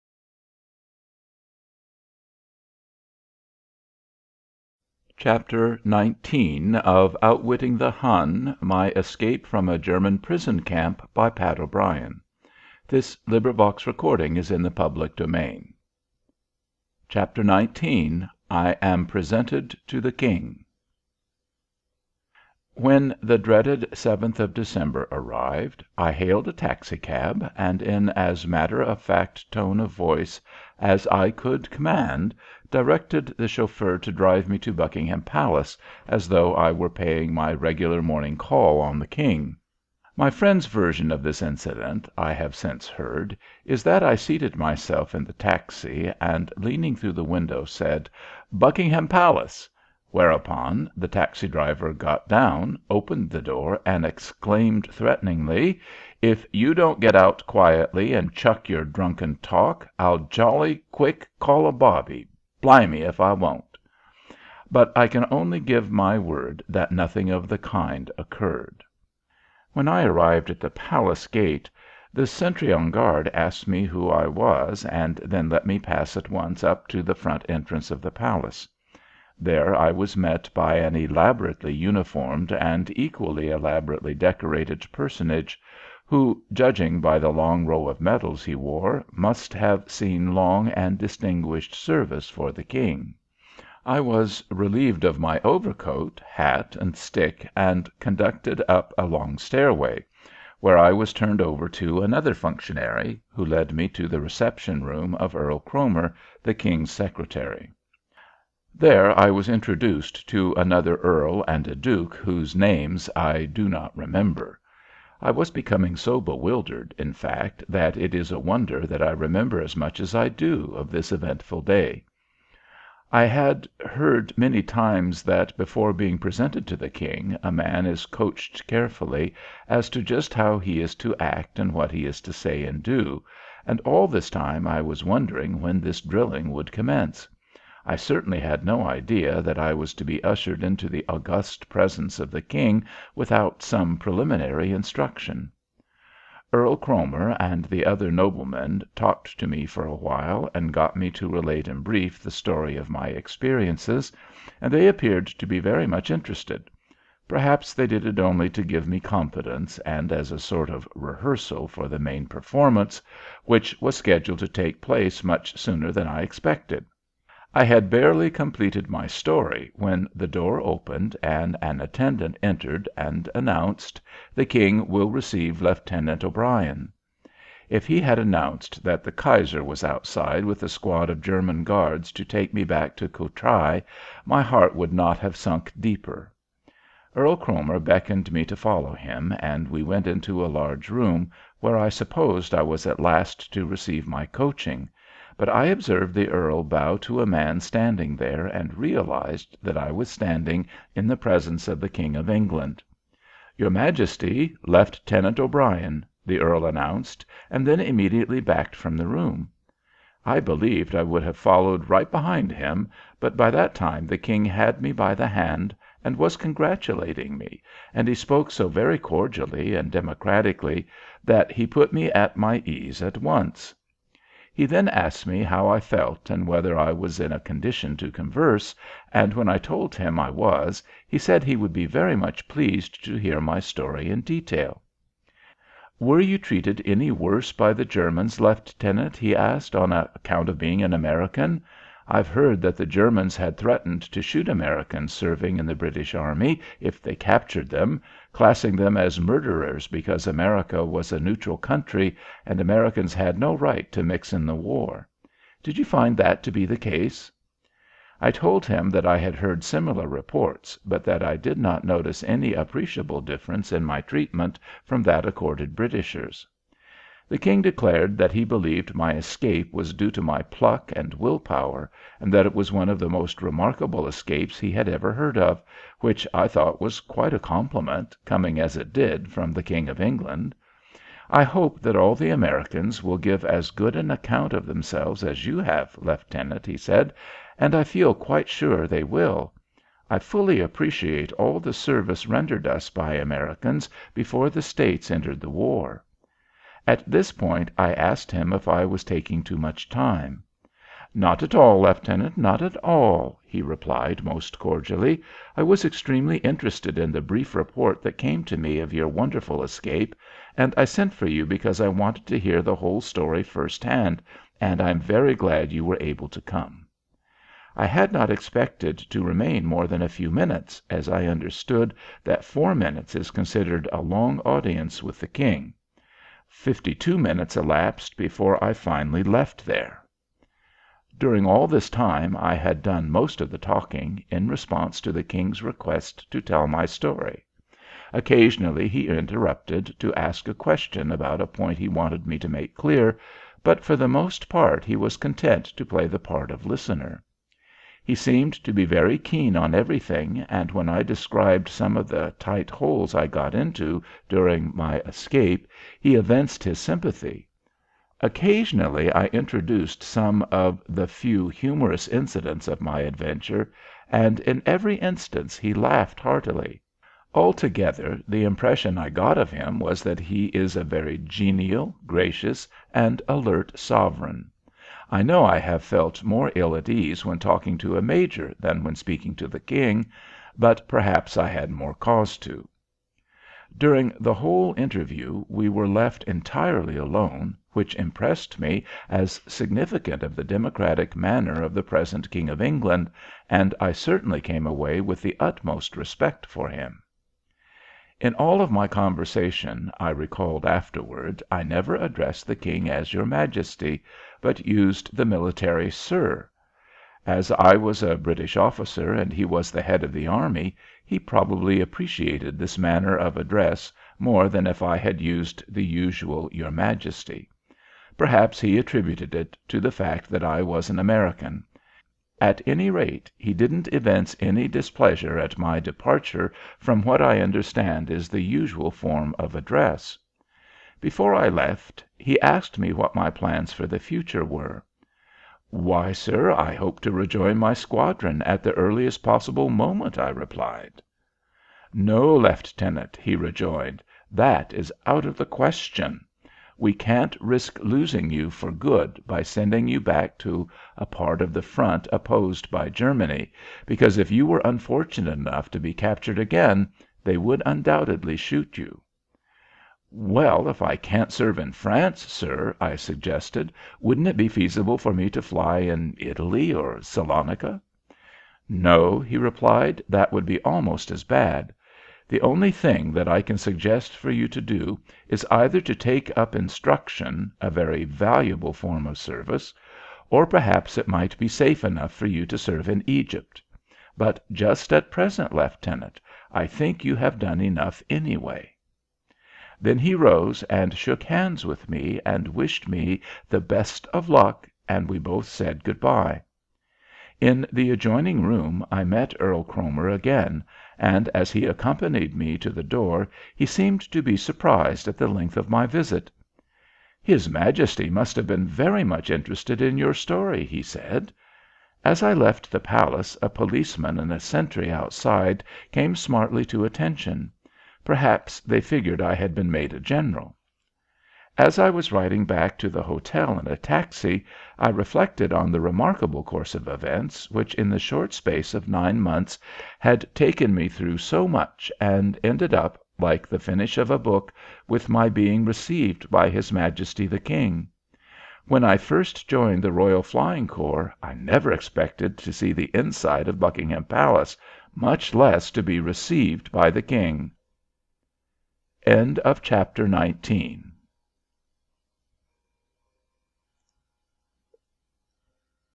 Outwitting the Hun; My Escape from a German Prison Camp by Pat O'Brien - 19. I Am Presented to...
LibriVox recording of Outwitting The Hun; My Escape From A German Prison Camp by Pat O'Brien. Read by David Wales.

Cover design by Kathryn Delaney. Cover illustrations from the book. This design is in the public domain.